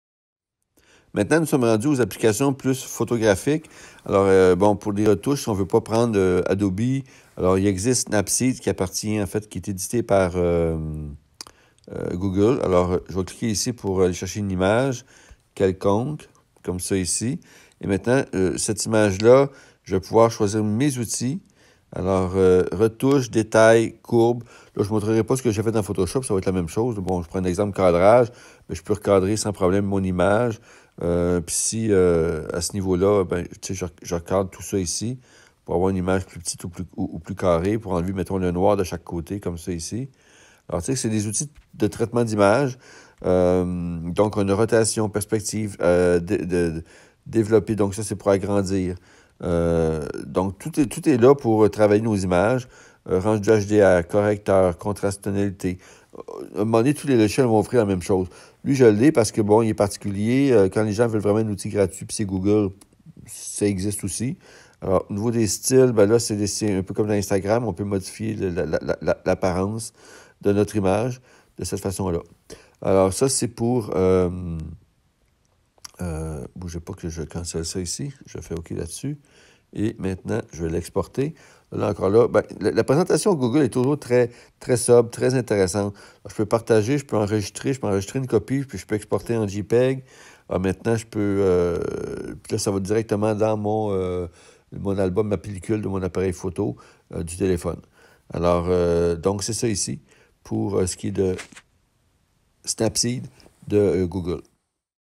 Google Snapseed
Cette apps est l'outils Google  pour de la retouche sur photographies.